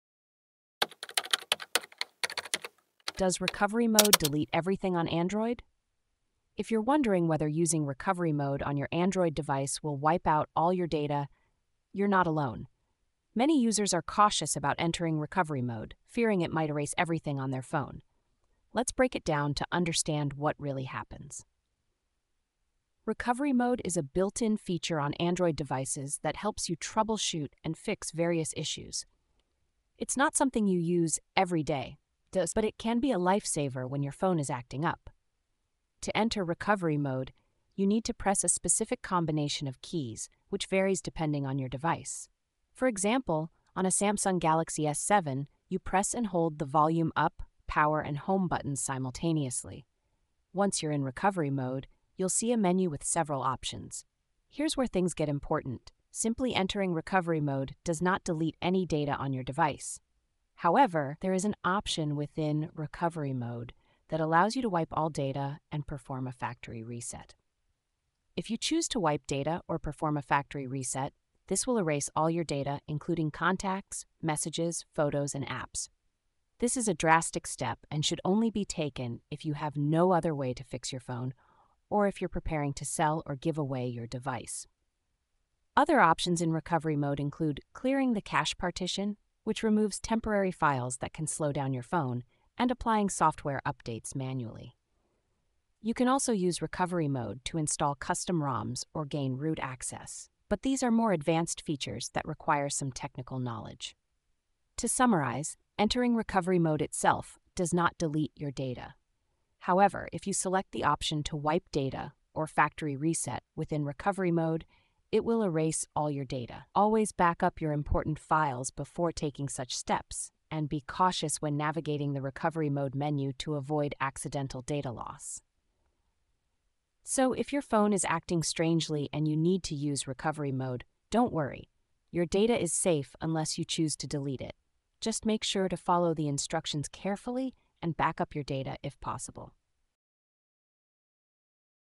Does Recovery Mode Delete Everything Android? - Be App Savvy
Does Recovery Mode Delete Everything
  Android? Have you ever been concerned about what happens when you enter Recovery Mode on your Android device? In this informative video, we’ll guide you through the ins and outs of Recovery Mode and its functions. We’ll explain how to access Recovery Mode on various Android devices and what options you'll encounter once you're there. Understanding Recovery Mode is essential, especially when your phone is facing issues, as it can help you troubleshoot effectively.

We’ll clarify the difference between entering Recovery Mode and the potential actions you can take that may affect your data. You’ll learn about the importance of backing up your files before making any changes and how to navigate the Recovery Mode menu safely. Additionally, we’ll touch on features such as clearing the cache partition and applying software updates, which can help improve your device's performance without risking your data.

Join us for this essential discussion to ensure you’re well-informed about Recovery Mode.